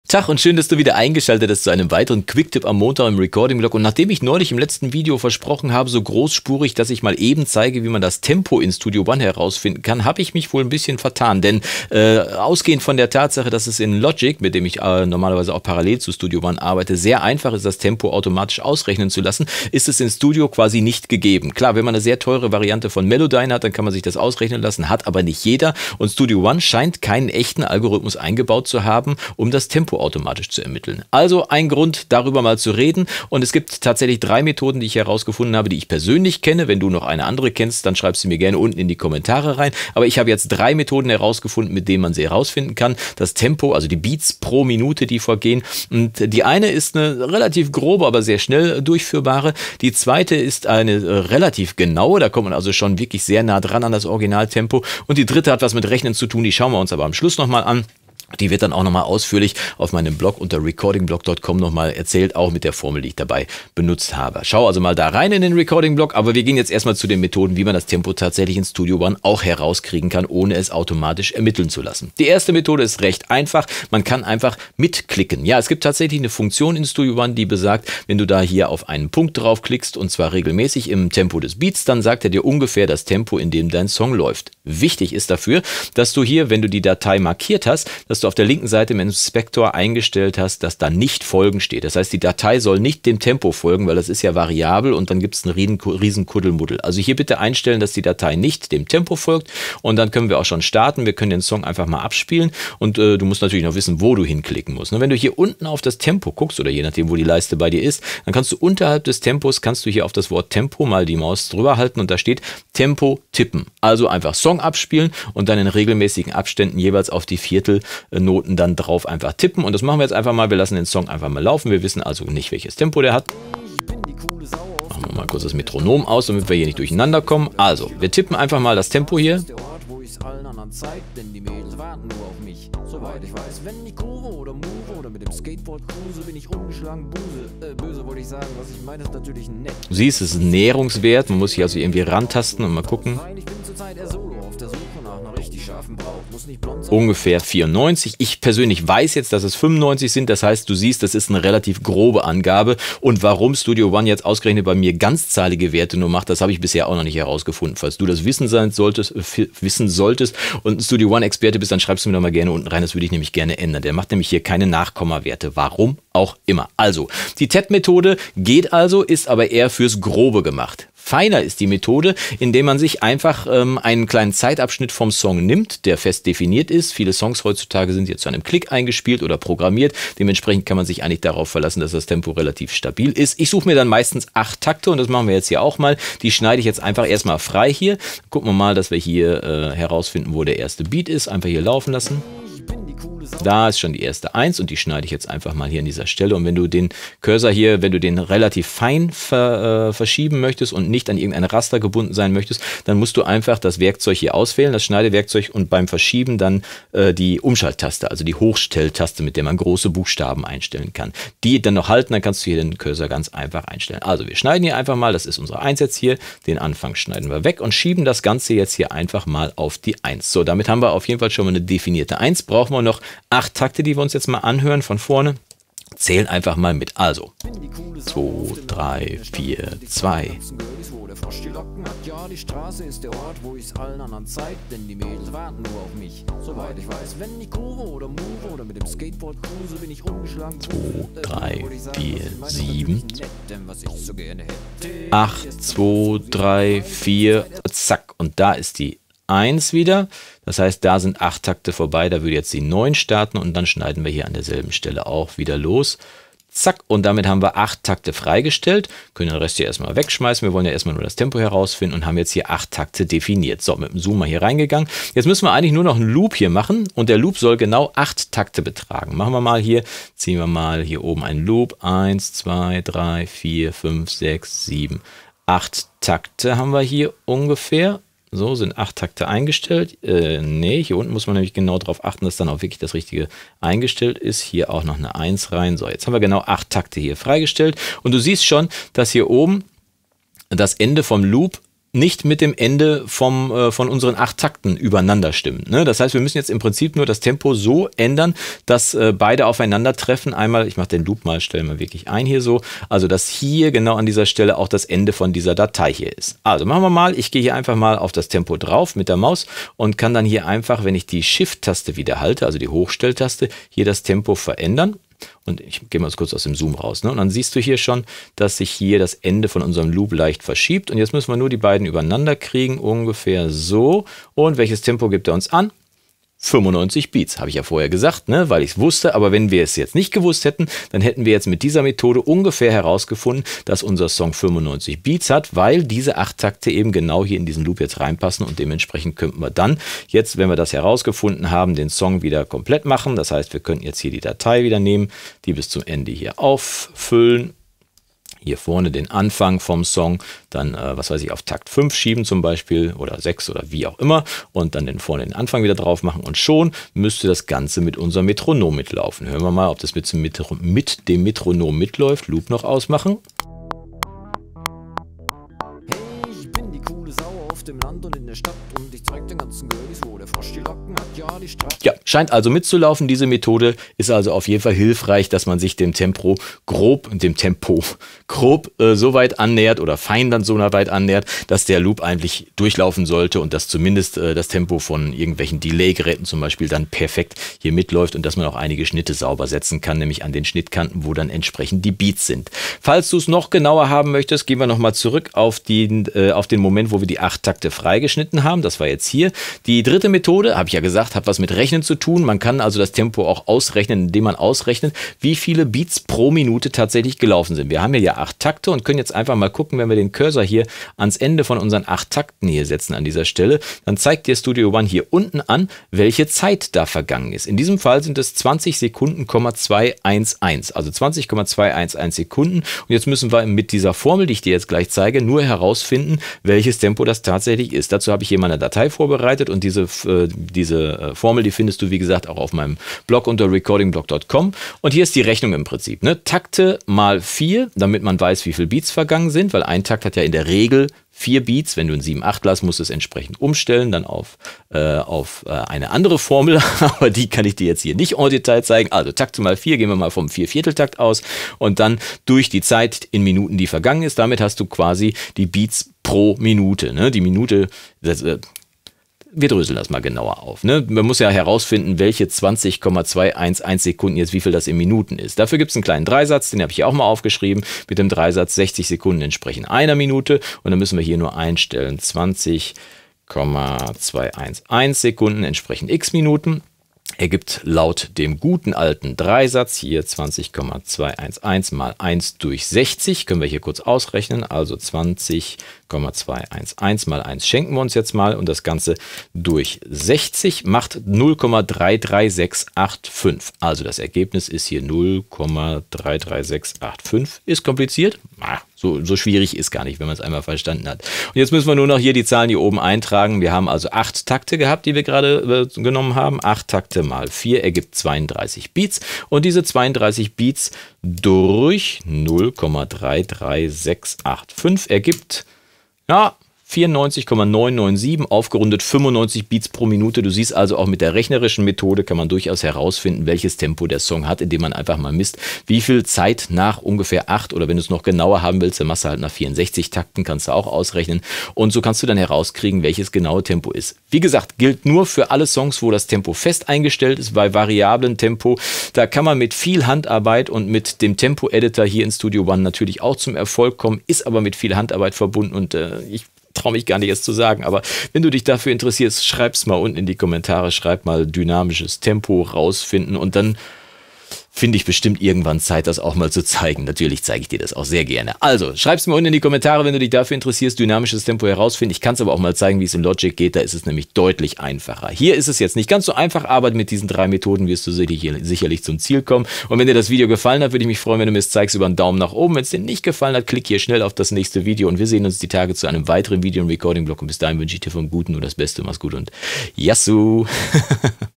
So klappt´s: Songtempo finden in Studio One 4 | Abmischen Tutorial | Recording-Blog Quicktip 61
Quicktip 61: Mal eben das Songtempo oder das Tempo eines Loops automatisch herausfinden. Klingt einfach, sollte es auch sein. Nur eben nicht in Studio One - zumindest nicht automatisch. Trotzdem gibt es 3 leichte Wege das Tempo selbst herauszufinden und die zeige ich Dir heute!